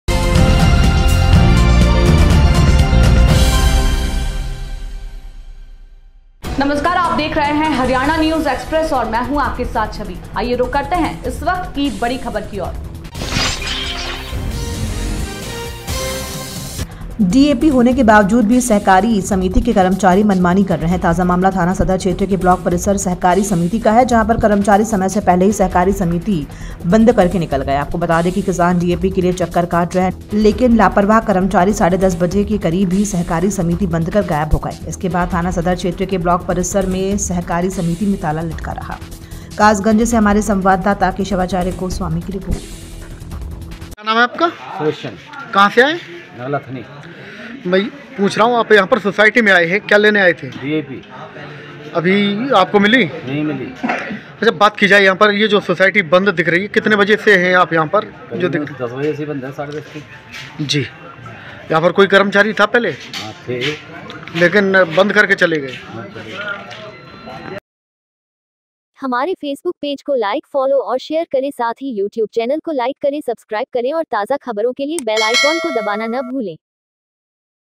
उत्तर प्रदेश : डीएपी होने के बावजूद भी सहकारी समिति के कर्मचारी कर रहे मनमानी
डीएपी होने के बावजूद भी सहकारी समिति के कर्मचारी मनमानी कर रहे हैं। ताजा मामला थाना सदर क्षेत्र के ब्लॉक परिसर की सहकारी समिति का है। जहां पर कर्मचारी समय से पहले ही सहकारी समिति बंद करके निकल गए। आपको बता दें कि किसान डीएपी के लिए चक्कर काट रहे हैं। लेकिन लापरवाह कर्मचारी साढ़े दस बजे के करीब ही सहकारी समिति बंद कर गायब हो गए। इसके बाद थाना सदर क्षेत्र के ब्लॉक परिसर में सहकारी समिति में ताला लटका रहा।